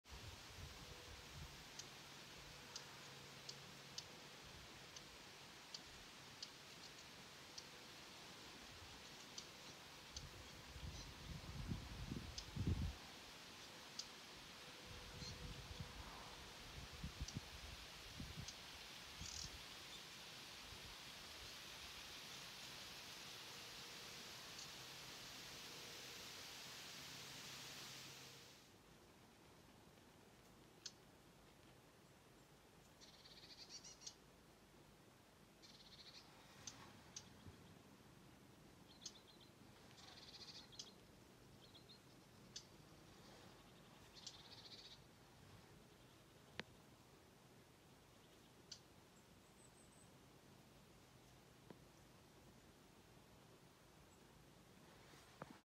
2023-07-22 Rörsångare knäppningar, Mingen Njurunda.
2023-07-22 Rörsångaren knäpper i vassen Mingen, Njurunda. Även en Blåmes stämmer in.